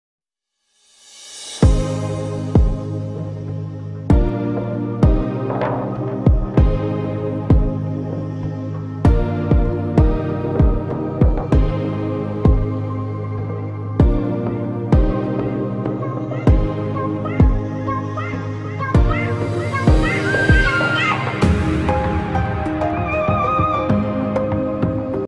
Alycia Baumgartner vs Edith Soledad Matthysse disrespect each other with Brutal Punch's to the face
Alycia Baumgartner vs Edith Soledad Matthysse disrespect each other with a Brutal Punch's to the face | Replay in Slow Mo